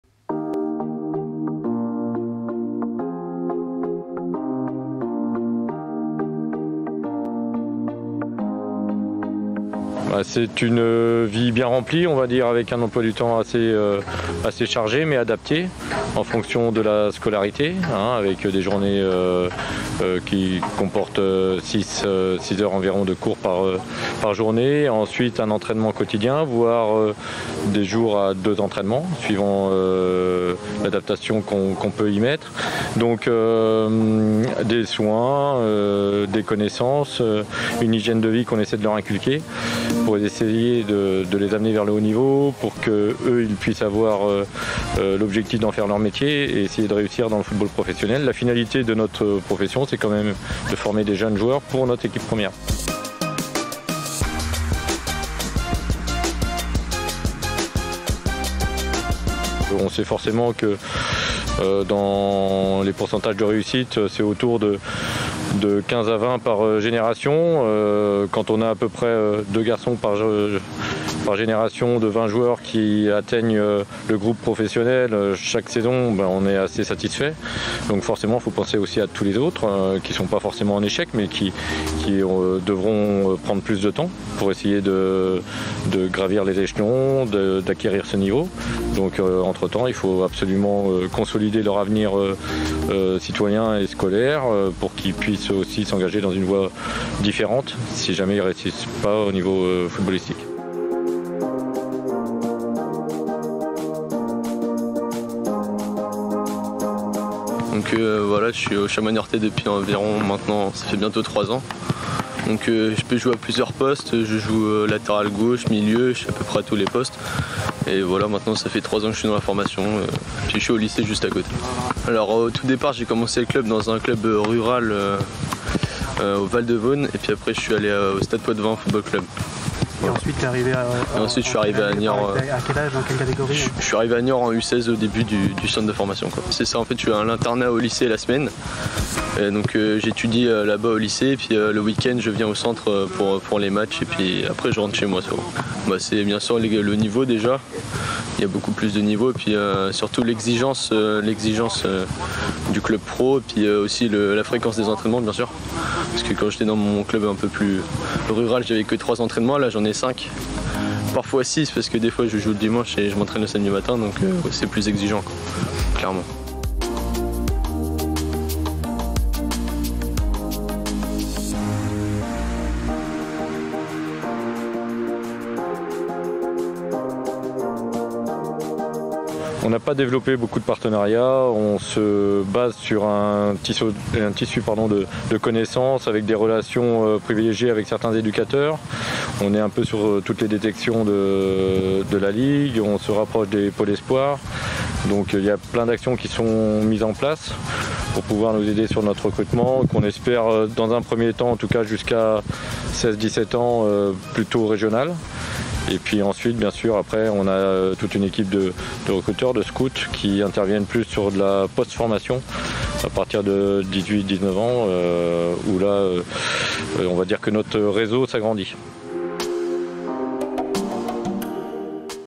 LA FORMATION AU CHAMOIS NIORTAIS FC
Le centre de formation est un élément essentiel de la plupart des clubs professionnel. À Niort, le Chamois Niortais ne déroge pas à la règle. Et l'aspect scolaire est aussi important que le sportif. Axel Toudic pour LFNA TV.